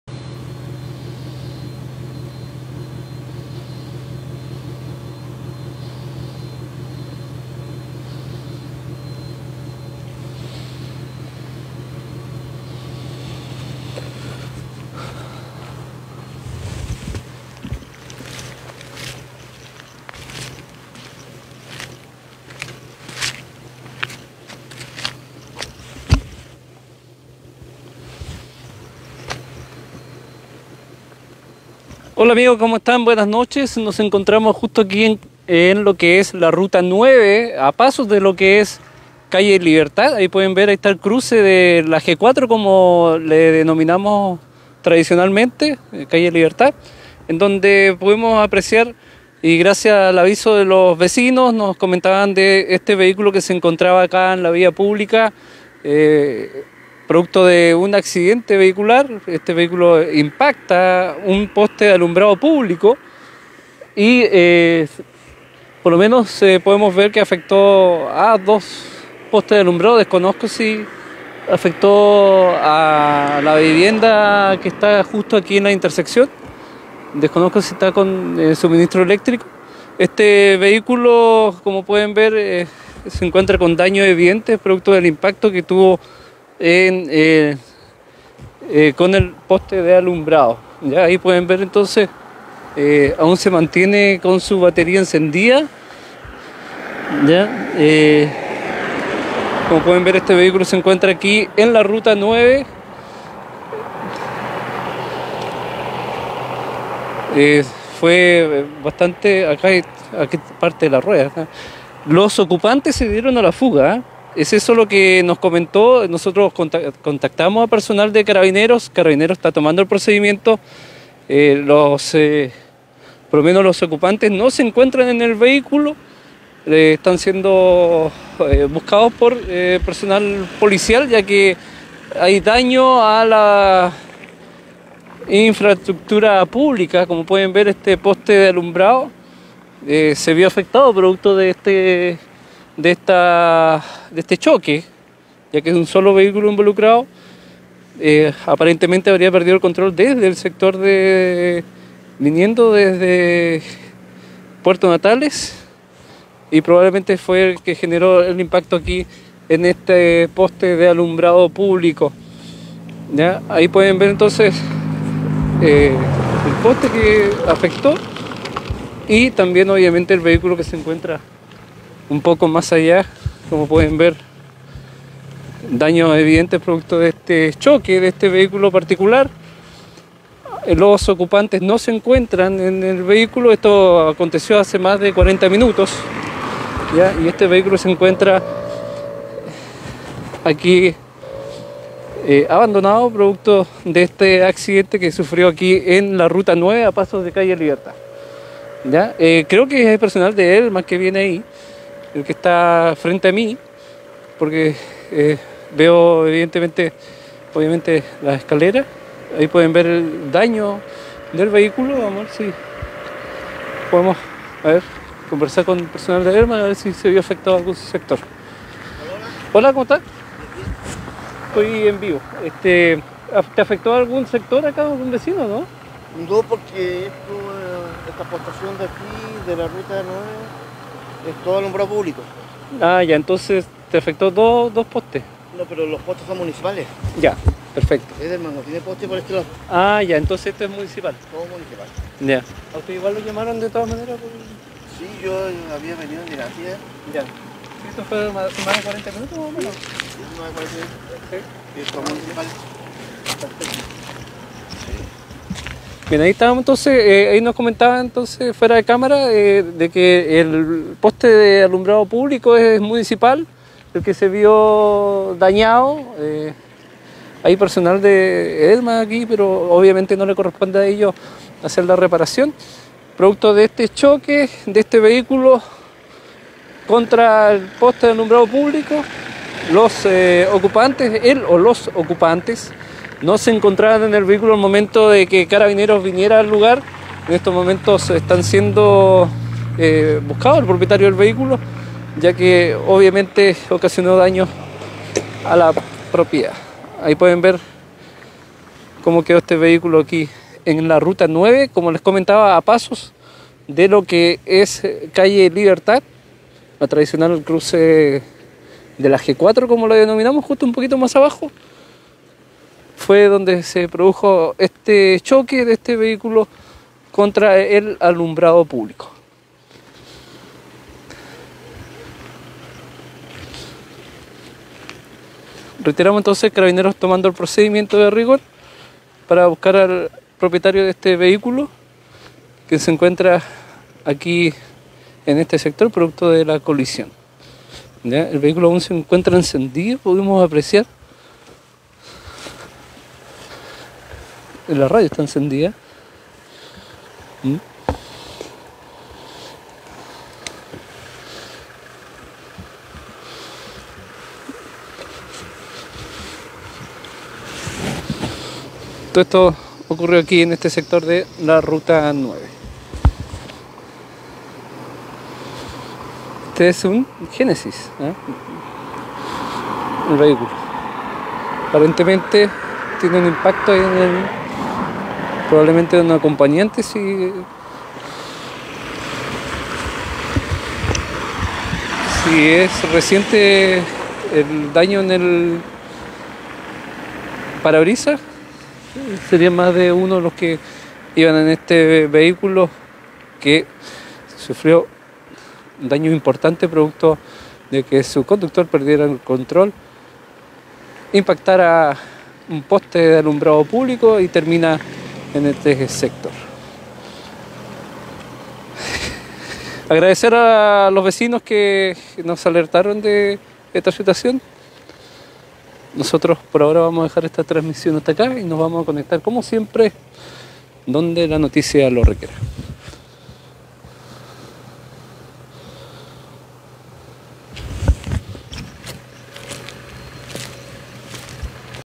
Conductor pierde el control de su vehiculo impactando un poste de alumbrado publico
Conductor pierde el control de su vehiculo impactando un poste de alumbrado público, al momento que Carabineros toma el procedimiento el o los ocupantes no se encuntraban en el lugar.